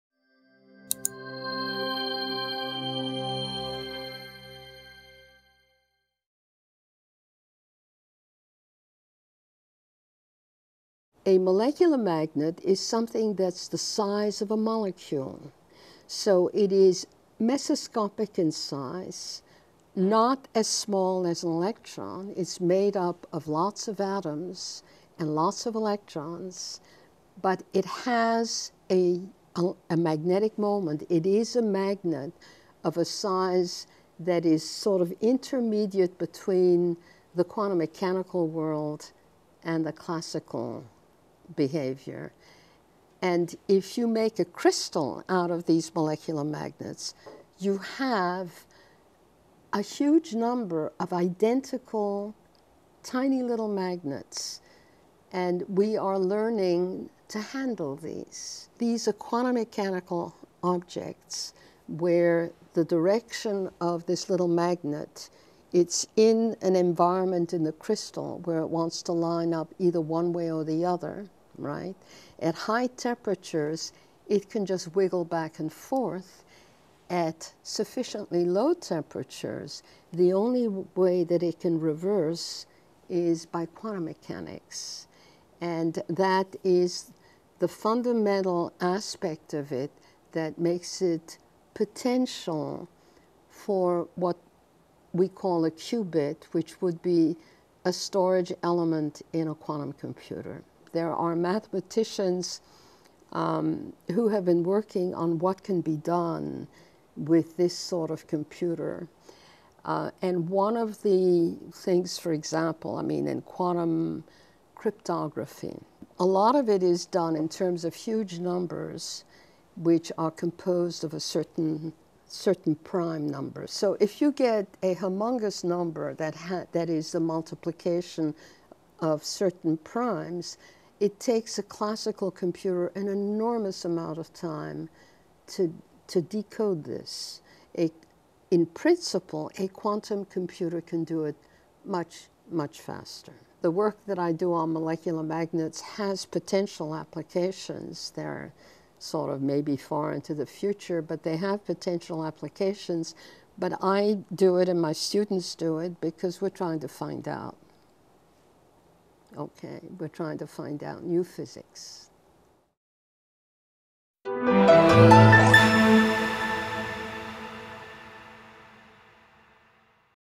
New Physics First
City College physicist Myriam Sarachik says the potential applications of her work with molecular magnets may be far in the future, but her  goal is "to find out new physics."